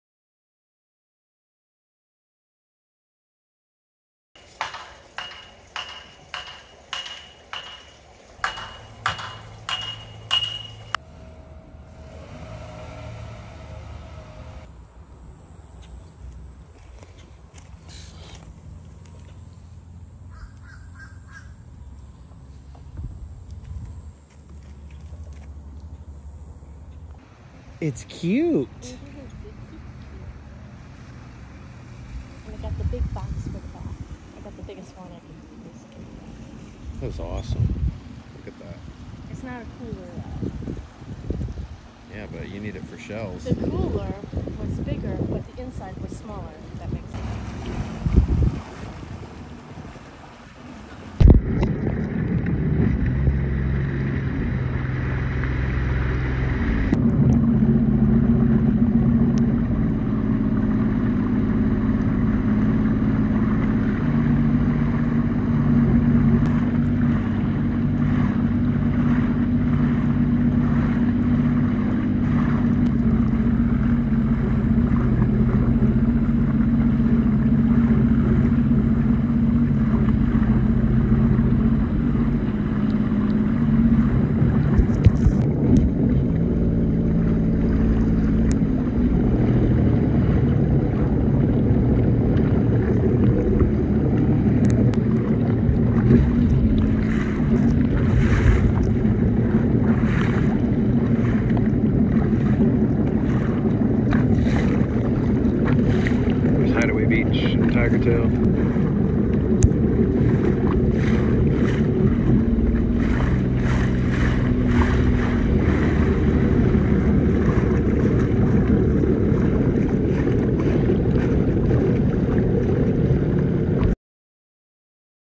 Meet Blue Pearl MWIP’S Mascot
Filmed in Marco Island, FL.

She is gassed up and ready to go on her first adventure with the Bob Squad. Special shoutout to Ruthie and Patrick for sharing this beauty with me.